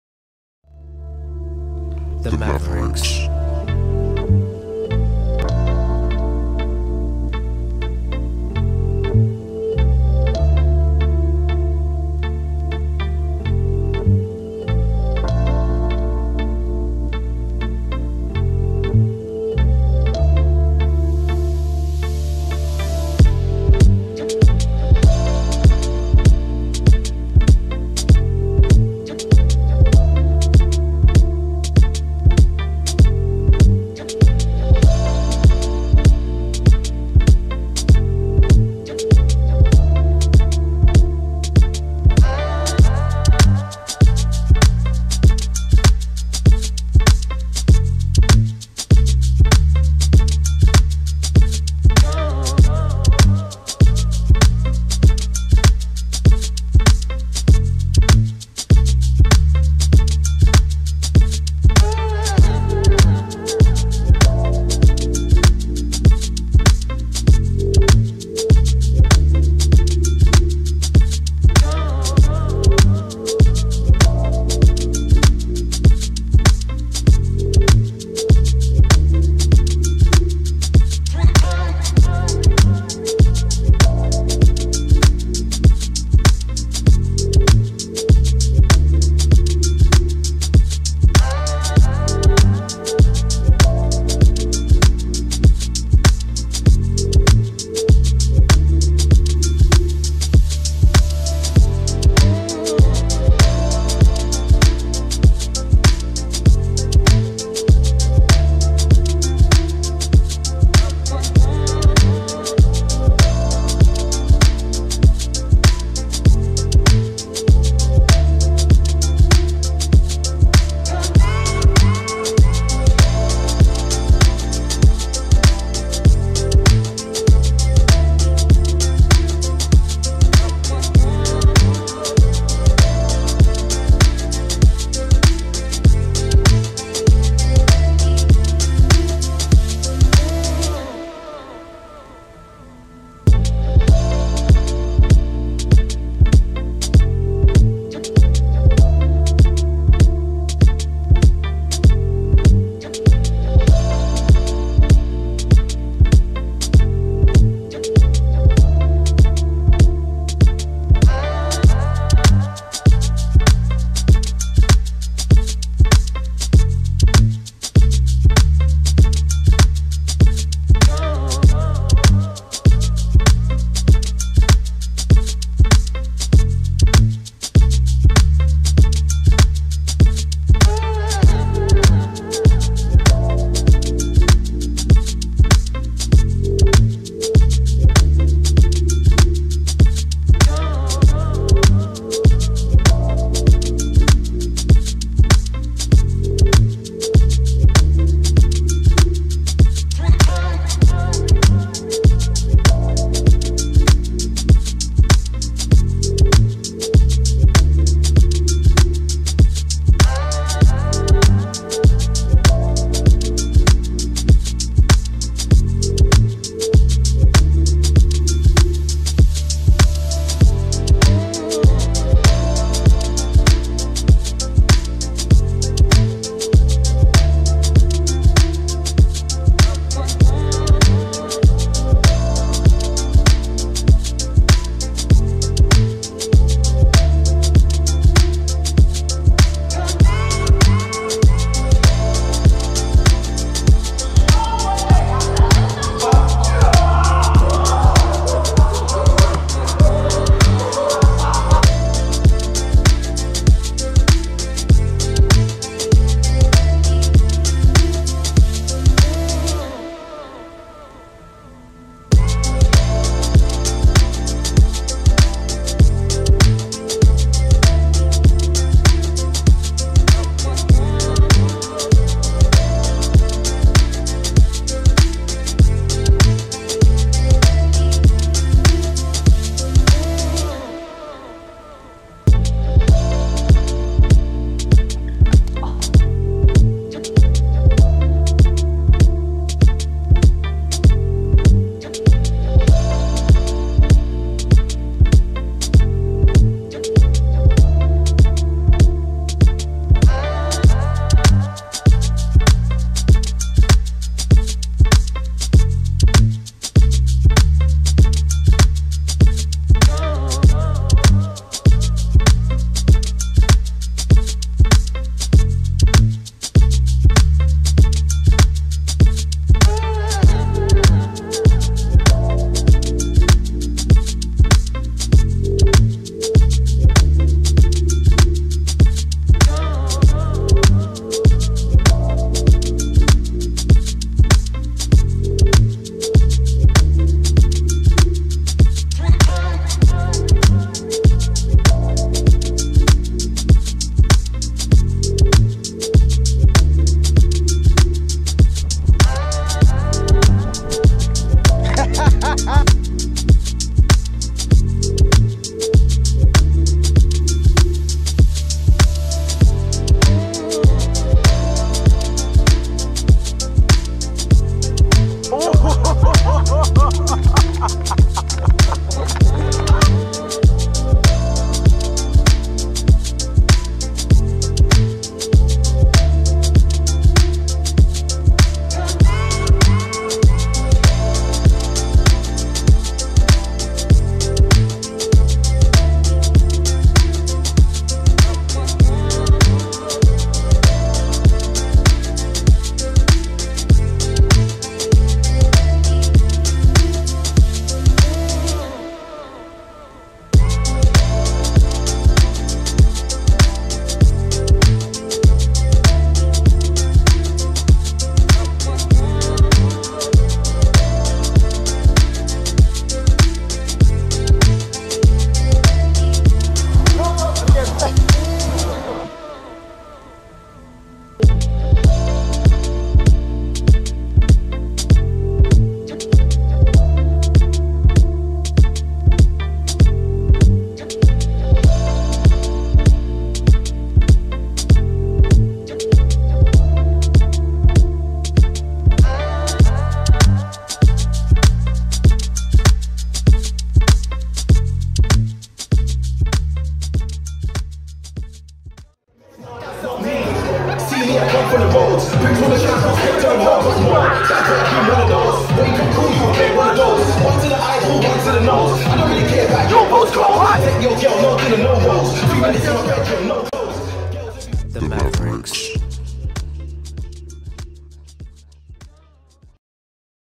SOUL MAVERICKS 2016 RECAP
This is a video tribute I made for the crew showing what we got up to this year. From the comps, to the shows, to the training and chilling together with friends all over the globe. Hope you enjoy the video.

You can also follow on Instagram: @spin167 and Facebook: Spin - Soul Mavericks